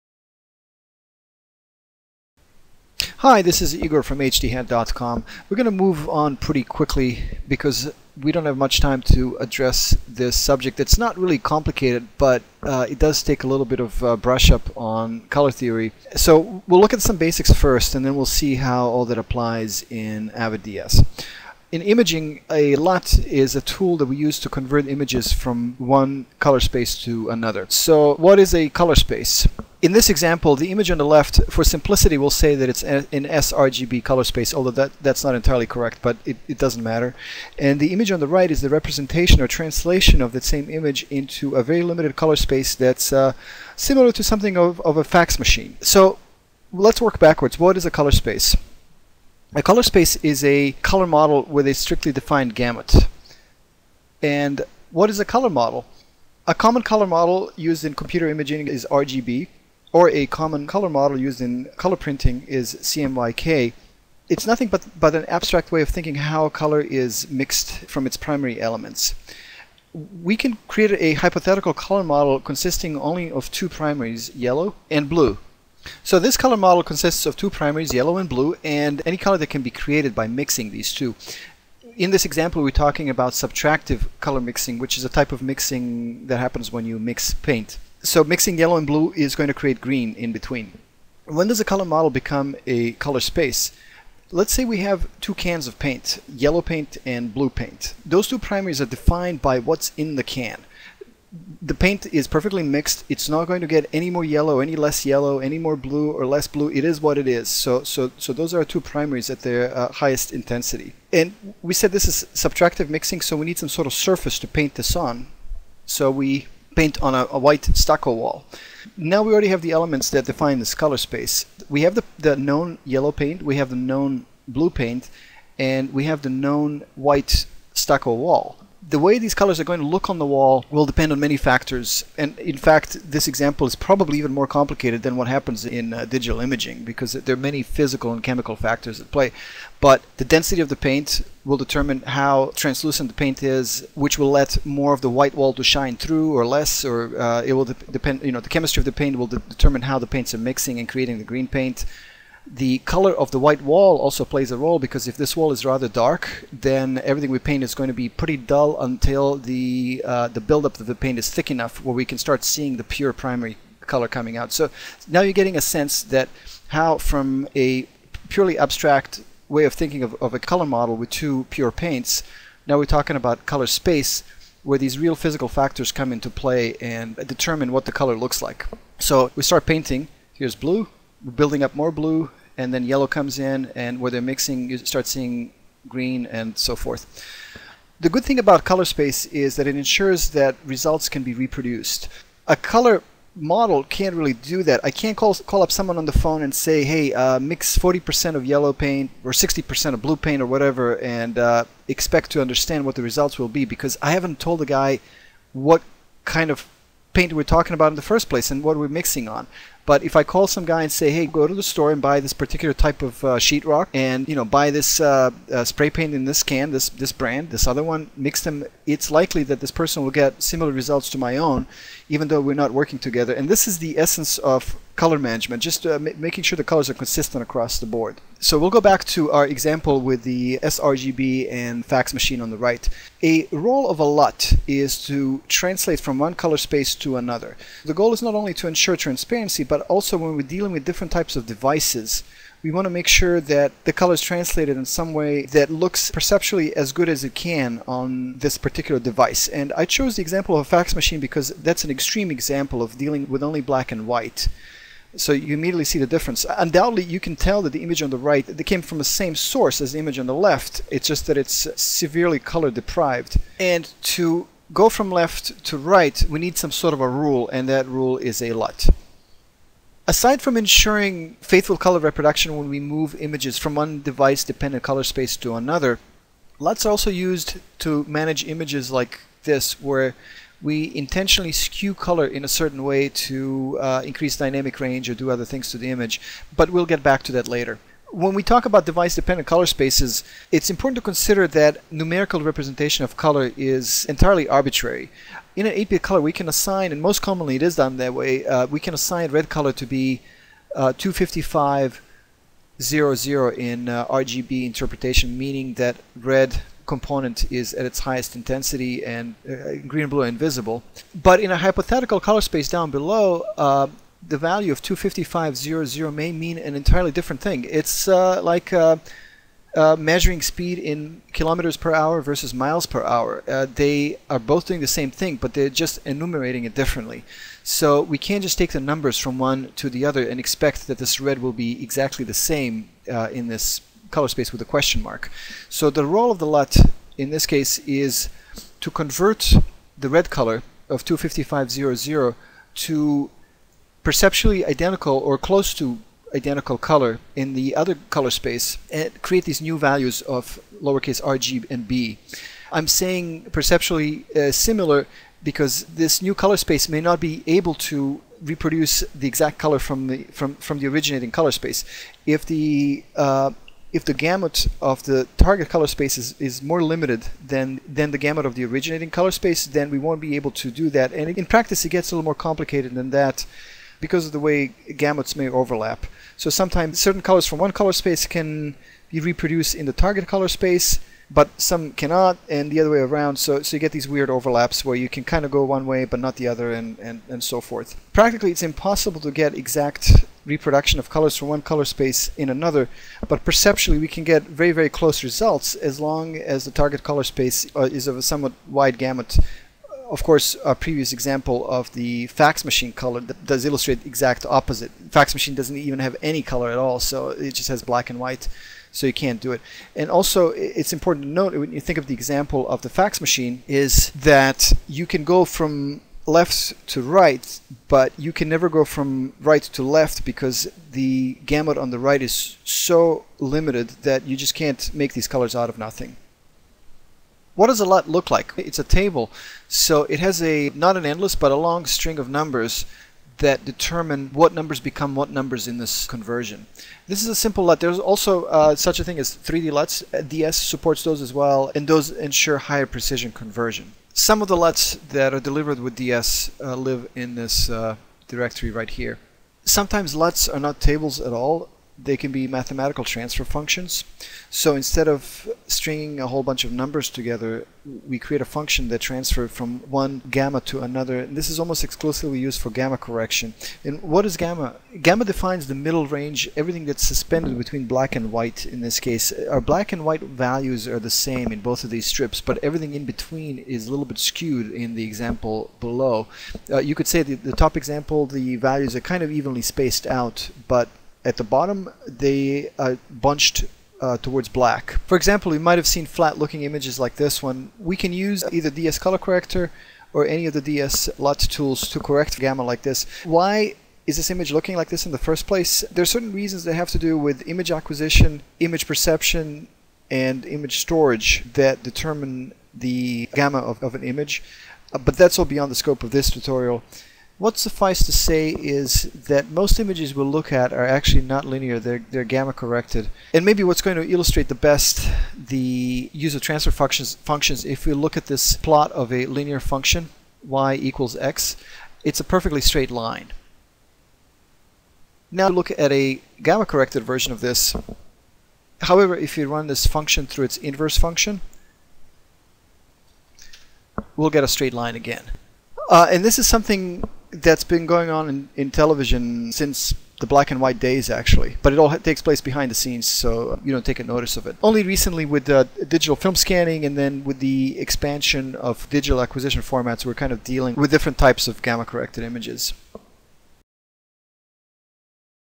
Avid DS Introduction to LUTs Part 1 of 2
The proliferation of digital camera formats over the last decade has introduced entirely new color management concepts into a once straightforward editorial color pipeline.

Although this tutorial by Igor Ridanovic from HDhead.com looks at Avid DS specific color management tools, it also lays down the basic groundwork necessary to understand lookup tables (LUTs) and their use in any other color managed environment.